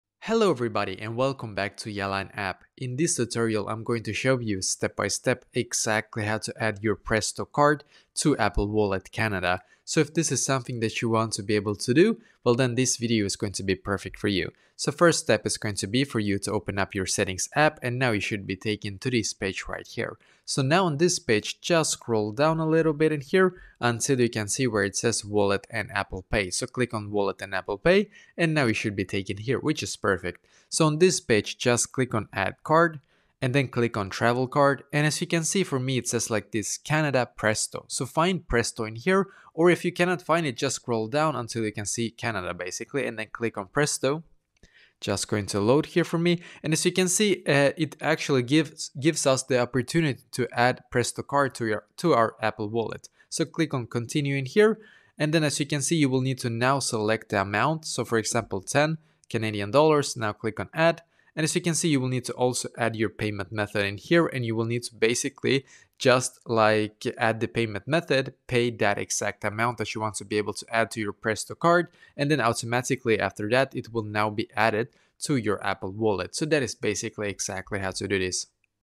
How To Add Presto Card To Apple Wallet Canada (2025)
If you want to be able to add a Presto card to Apple Wallet in Canada, then this video will be perfect for you!